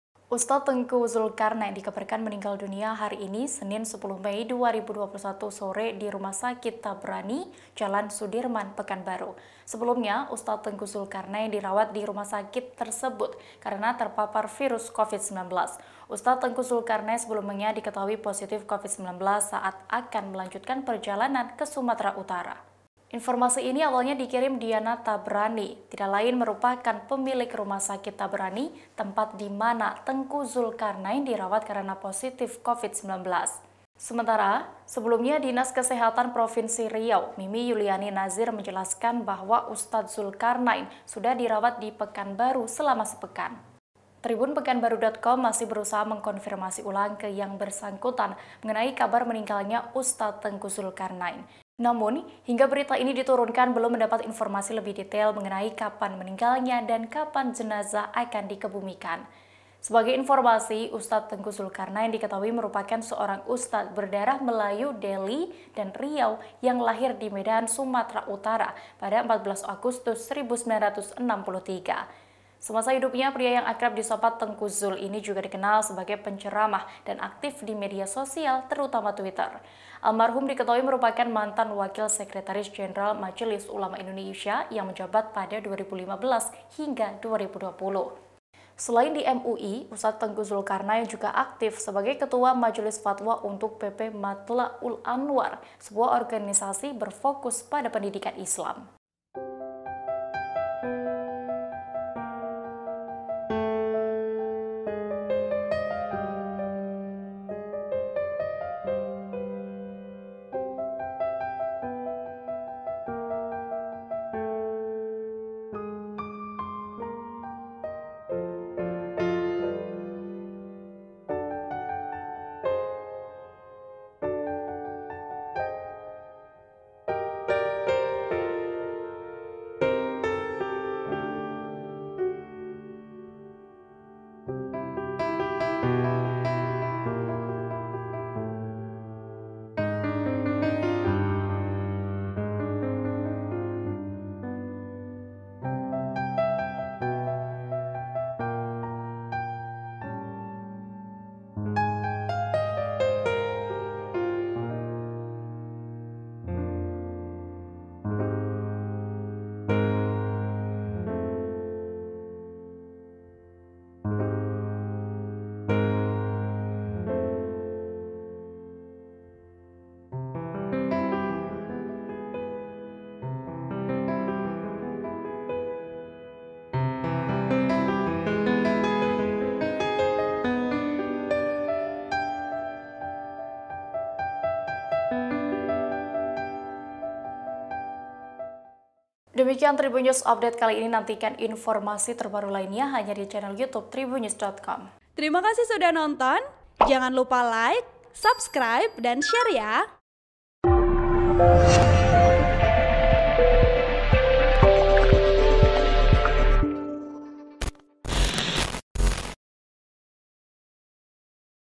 Pendakwah Ustaz Tengku Zulkarnain Meninggal Dunia saat Jalani Perawatan karena Terpapar Covid-19
TRIBUN-VIDEO.COM - Ustaz Tengku Zulkarnain dikabarkan meninggal dunia hari ini, Senin (10/5/2021) sore, di RS Tabrani, Jalan Sudirman, Pekanbaru.

Sebelumnya, Ustaz Tengku Zulkarnain dirawat di rumah sakit tersebut karena terpapar virus Covid-19.

Ustaz Tengku Zulkarnain sebelumnya diketahui positif COVID-19 saat akan melanjutkan perjalanan ke Sumatera Utara (Sumut).

Informasi ini awalnya dikirim Diana Tabrani yang tidak lain merupakan pemilik rumah sakit Tabrani tempat dimana Tengku Zulkarnain dirawat karena positif covid-19.

"Innalillahi wa innailaihi rooji'un. Telah berpulang ke Rahmatullah Ust. Tengku Zulkarnain. Semoga amal ibadah beliau diterima Allah SWT."

"Semoga keluarga yang ditinggalkan diberikan kesabaran dan ketabahan." Demikian postingan Diana Tabrani di satu group pertemanan WhatsApp.

Kepala Dinas Kesehatan Provinsi Riau Mimi Yuliani Nazir menjelaskan bahwa Ustaz Zulkarnain sudah dirawat di Pekanbaru selama sepekan.

"Beliau sudah dirawat sejak tanggal 3 Mei 2021," kata Mimi yang juga Koordinator Satgas Covid 19 Riau, Senin (10/5/2021).

Tribunpekanbaru.com, masih berusaha mengkonfirmasi ulang ke yang bersangkutan mengenai kabar meninggalnya Ustaz Tengku Zulkarnai.

Namun, hingga berita ini diturunnya belum mendapat informasi lebih detail mengenai kapan meninggal dan kapan jenazah akan dikebumikan.

Sebagai informasi, Ustaz Tengku Zulkarnain diketahui merupakan seorang ustaz berdarah Melayu Deli dan Riau yang lahir di Medan, Sumatra Utara, 14 Agustus 1963.

Almarhum diketahui merupakan mantan Wakil Sekretaris Jenderal Majelis Ulama Indonesia (MUI) yang menjabat pada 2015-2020.

Selain di MUI, Ustaz Tengku juga aktif sebagai Ketua Majelis Fatwa untuk PP Mathla'ul Anwar, sebuah organisasi berfokus pada pendidikan Islam. (Tribun-video.com/TribunPekanbaru.com)

Artikel ini telah tayang di TribunPekanbaru.com dengan judul BREAKING NEWS: Ustadz Tengku Zulkarnain Dikabarkan Meninggal Dunia, Sebelumnya Positif Covid-19,
Penulis: Nasuha Nasution
Editor: Muhammad Ridho